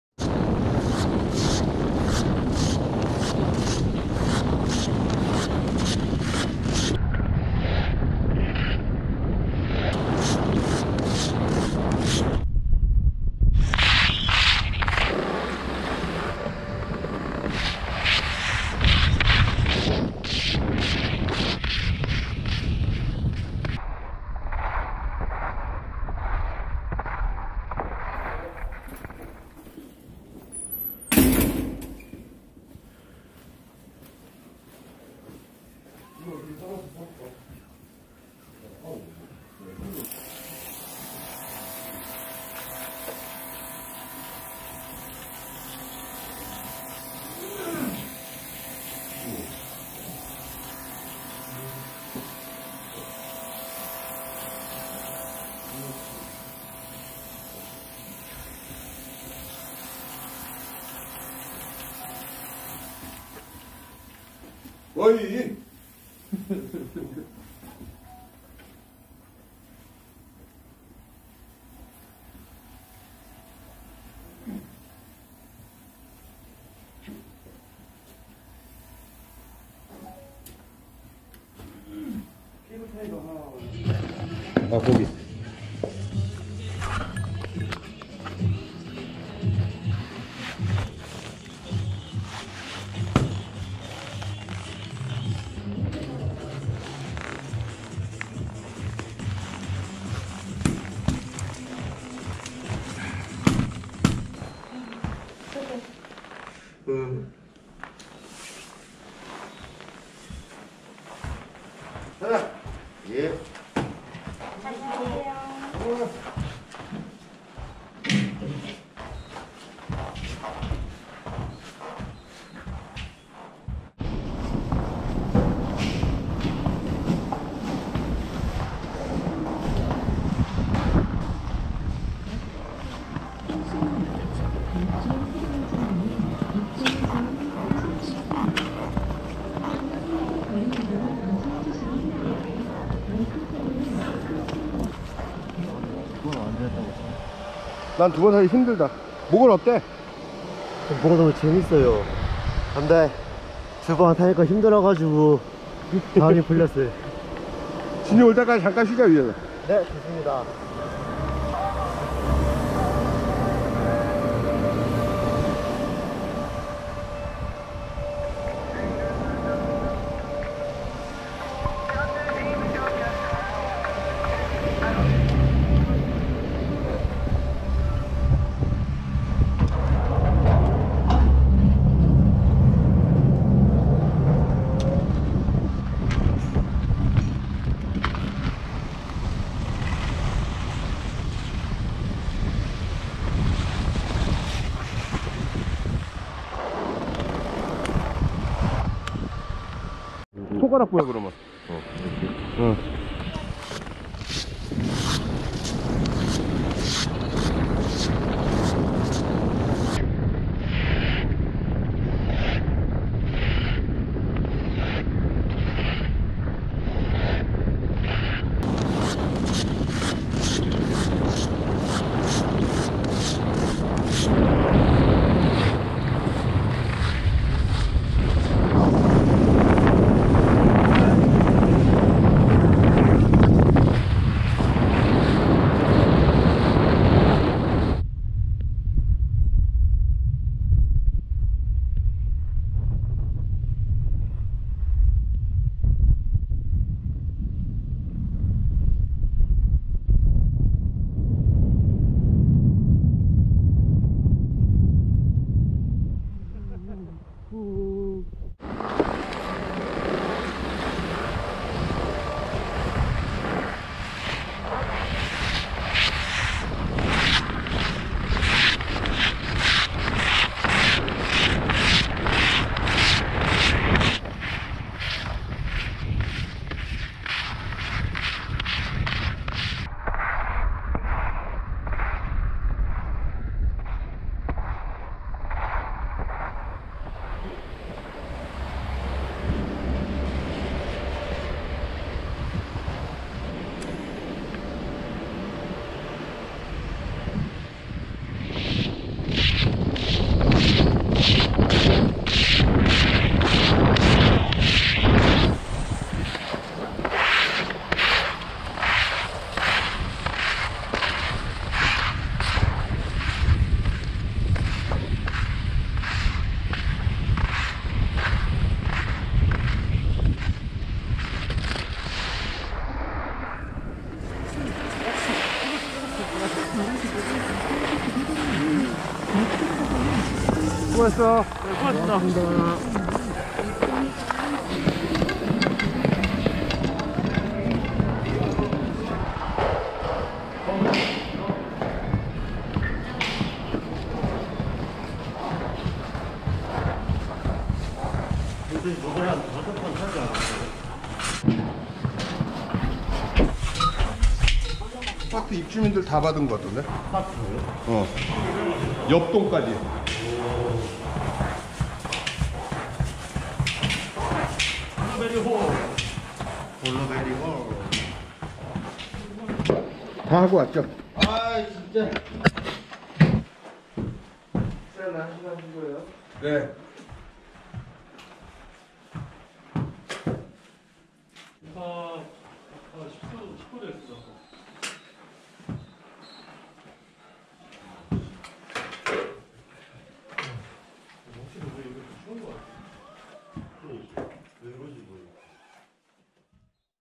웰리힐리 파크 김태일 모글 스쿨 2020 12 16 B2 모글 스킹
오랜만에 고프로 모글 스킹 촬영.
모글 사무실 1일 2회 코로나 자체 방역 실시.
건강한 거울을 위해 마스크 필수. 개인 위생 철저히~